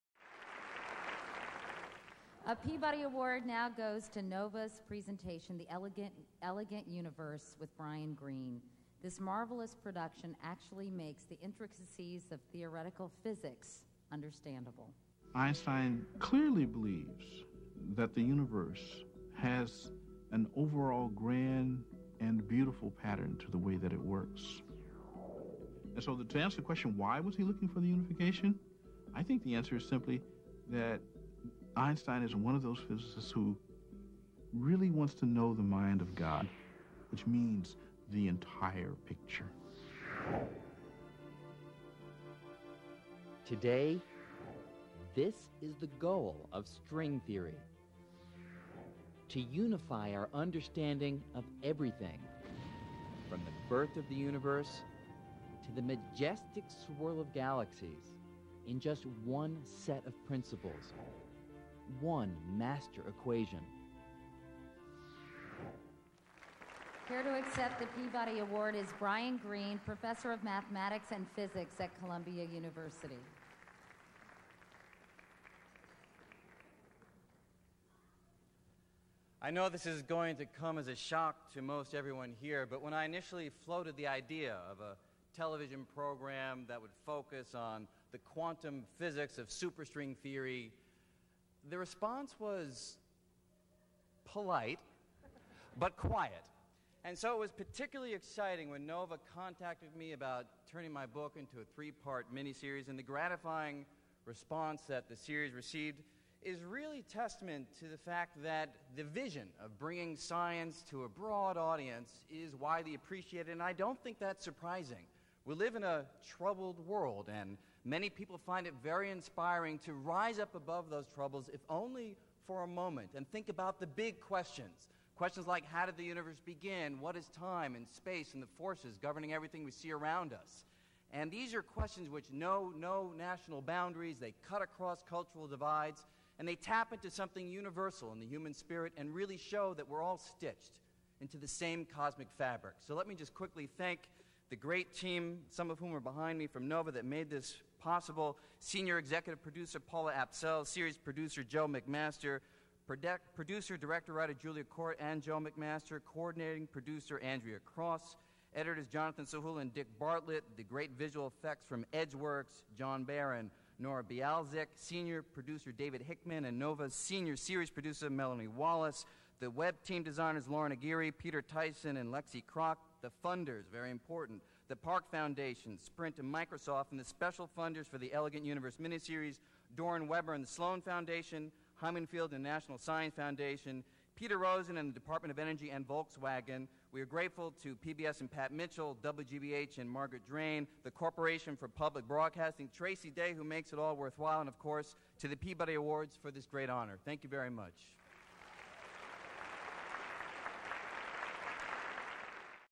Brian Greene - NOVA: The Elegant Universe - 2003 Peabody Award Acceptance Speech
Winner 2003 | NOVA/WGBH, Channel 4, in association with David Hickman Films, Sveriges Television and Norddeutscher Rundfunk

The Elegant Universe with Brian Greene, based on the acclaimed physicist’s best-selling book, explores science’s most elaborate and ambitious theory, the string theory. Preparing such a topic—little of which can be seen or filmed—for television was equally ambitious. Yet host Brian Greene makes the abstract concrete, the complicated clear, and the improbable understandable in this entertaining yet sophisticated program. Blending factual story telling with animation, special effects, and trick photography, the producers turn the universe upside down and inside out in their quest to explain the fundamental laws of the science. A rich accompanying Web site enables users to deepen and apply their knowledge and explore ideas with interactive games and demonstrations. Also available are a downloadable version of the complete series and an excellent teacher’s guide complete with activities, puzzles, and resources for further study. This program and its related Web site may have opened the world of physics to the largest number of television viewers ever to approach the topic. The Elegant Universe with Brian Greene was written, produced and directed by Joseph McMaster and Julia Cort and edited by Jonathan Sahula and Dick Bartlett. Paula S. Apsell served as executive producer, with David Hickman as series producer. Lauren Aguirre served as executive editor for the Web site. Peter Tyson served as editor in chief, with senior web designer Anya Vinokou and senior web developer Brenden Kootsey. The program was produced for WGBH/Boston and Channel 4 in association with David Hickman Films, Sveriges Television, and Norddeutscher Rundfunk. A Peabody goes to The Elegant Universe with Brian Greene, for an amazing ride through the wonders and possibilities of our universe.